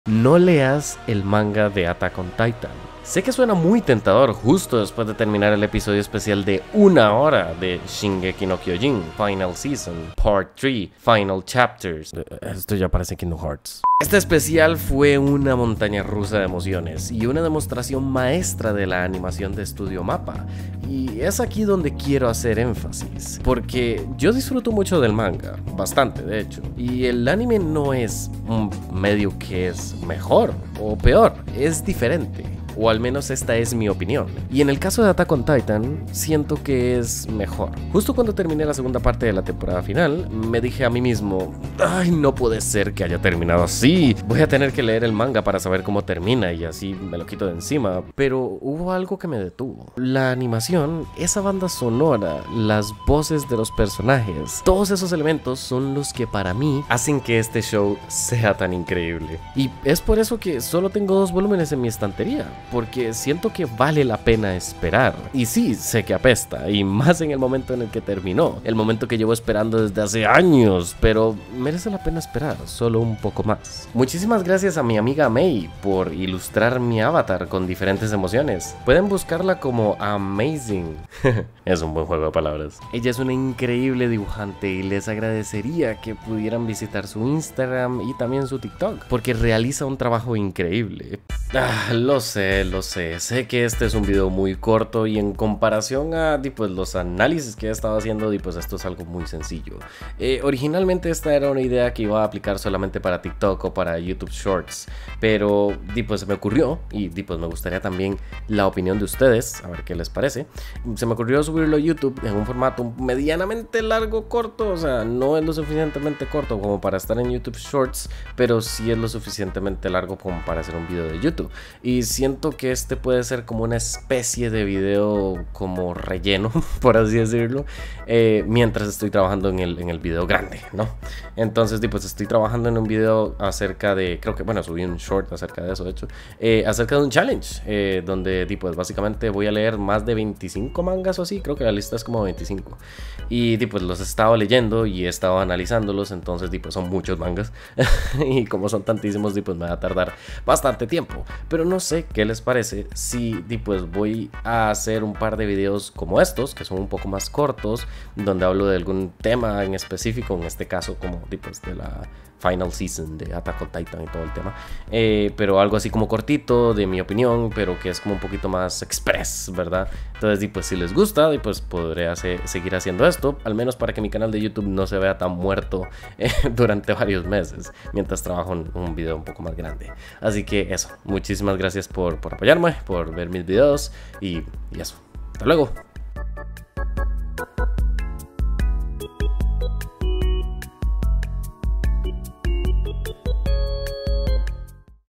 NO leas el manga de Attack On Titan
🎨 Echale un vistazo al arte de Mey!

Definitivamente esta es una de las instacias donde la paciencia es LA MEJOR OPCION!
Espero que te haya gustado mucho el video, sé que es un video un poco mas pequeño de lo normal pero pronto estaré posteando un video de verdad T_T
pero de momento estamos en el arco del relleno XD